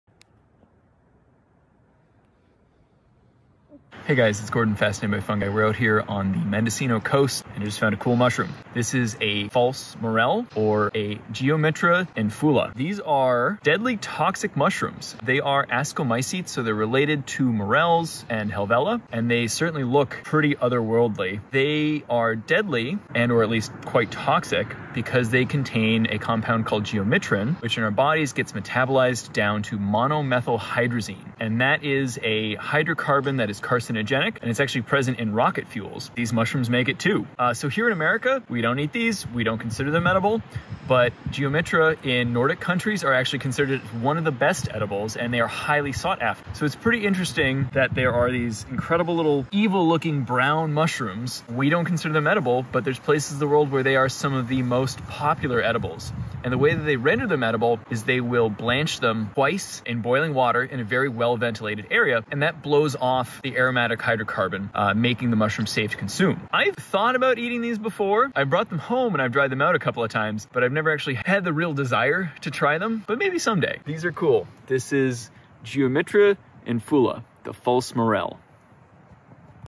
Identifying the toxic False Morel (Gyromitra infula) in Mendocino, CA
In the mad rush for Morels, other Ascomycetes often temp hunters as FalseMorels, while several genera and species are confused for true Morchella, Gyromitra are by far the most problematic. My best guess for this species is either Gyromitrainfula or possibly Gyromitraambigua. These species are quite similar, with infula being lighter brown and ambigua being more violet/purple brown and a more northern distribution (according to Michael Kuo). They are both saprobic, growing out of dead wood or terrestrially (as these specimens were).
Gyromitra contain gyromitrin, which is metabolized into the carcinogenic Monomethylhydrazine in our bodies. If consumed these mushrooms can make you quite sick, with MMH inducing severe gastric upset and other symptoms of poisoning. These types of mushrooms can however be rendered edible, and are especially prized in Scandinavian countries. A double boil in a well ventilated space helps to blow off the MMH. I was just happy to find some cool Ascomycetes up in Mendocino.